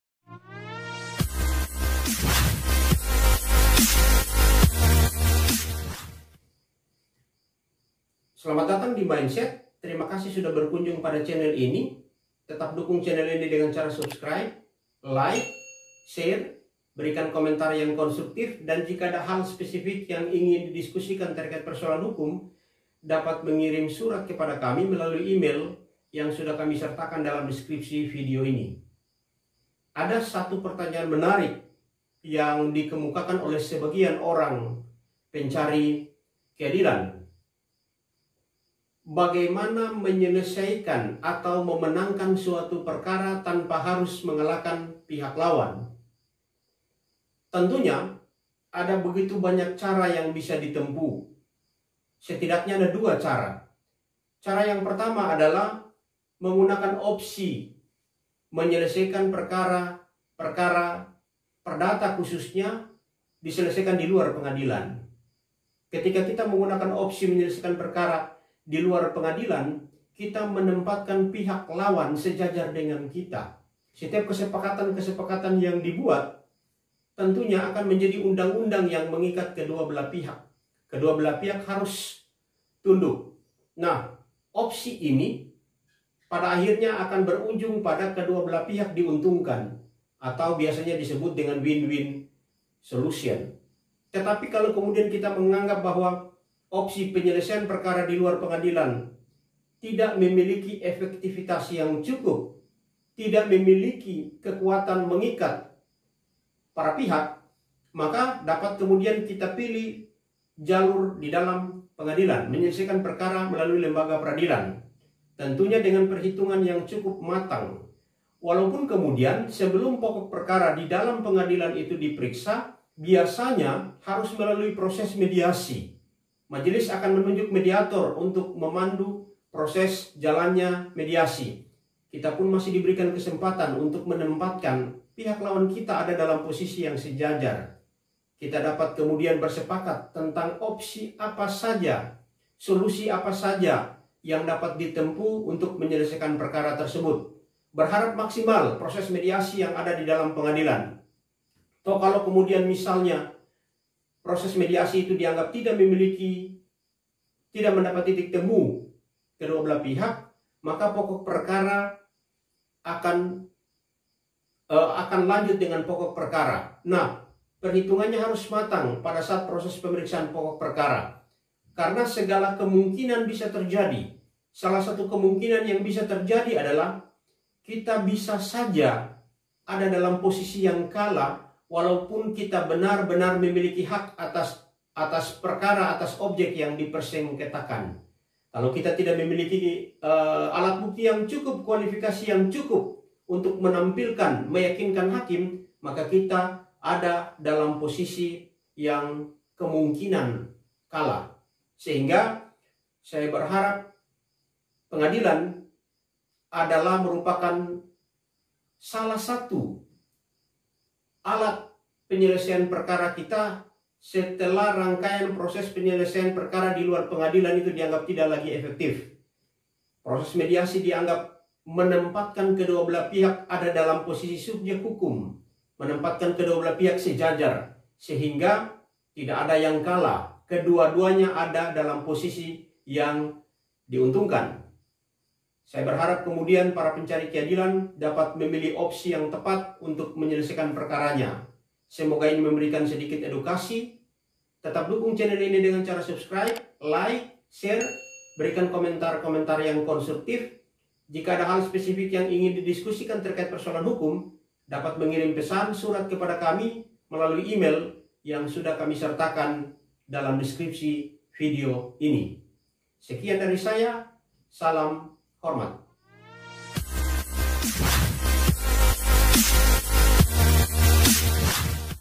Agnus Pasaribu: Memenangkan Perkara Tanpa Mengalahkan
Memenangkan Perkara tanpa mengalahkan lawan adalah cara ideal yang dipilih orang bijak.